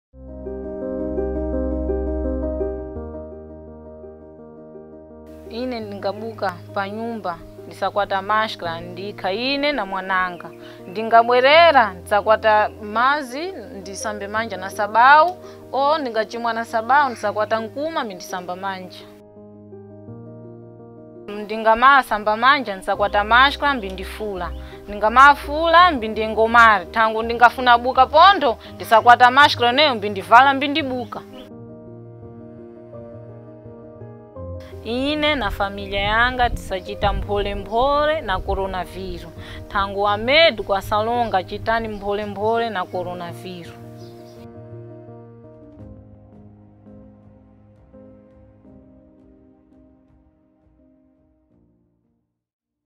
StopCovid19 boas práticas em xiSena
A dona de casa, Maria Elisa, partilha como ela e sua família se previnem da covid-19. Falando na língua xiSena, mostra como medidas locais simples podem ser adoptadas.

A campanha de prevenção e resiliência comunitária contra o novo coronavírus é liderada pelo CDD e CESC.